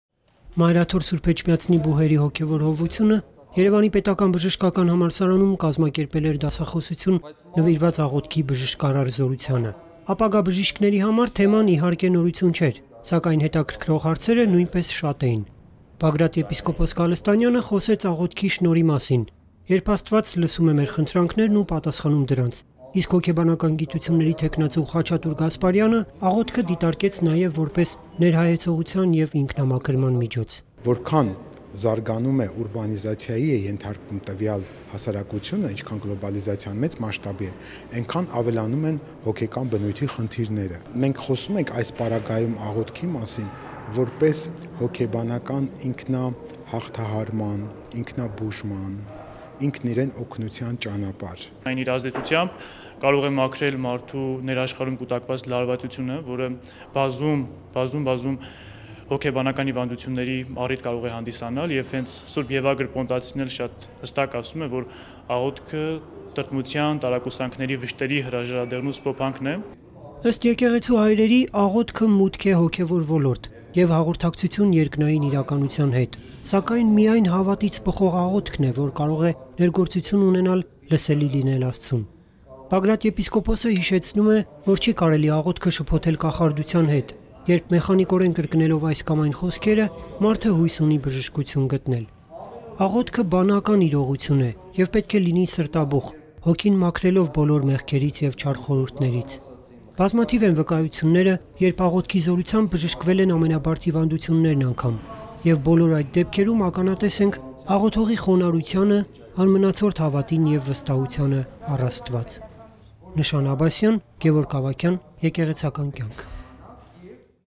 Lecture on the Prayer
The College and University Program of the Mother See of Holy Etchmiadzin has recently initiated a lecture on the healing power of prayer . His Grace Bishop Bagrat Galstanyan spoke on the topic at the Mkhitar Heratsi Yerevan State Medical University.

Դասախոսություն՝ աղոթքի մասին

Մայր Աթոռ Սուրբ Էջմիածնի Բուհերի հոգեւոր հովվությունը Երևանի՝ Մխիթար Հերացու անվան պետական բժշկական համալսարանում կազմակերպել էր դասախոսություն` նվիրված աղոթքի բժշկարար զորությանը: Այս թեմայով դասախոսություն կարդաց Գերշ. Տ. Բագրատ եպս. Գալստանյանը: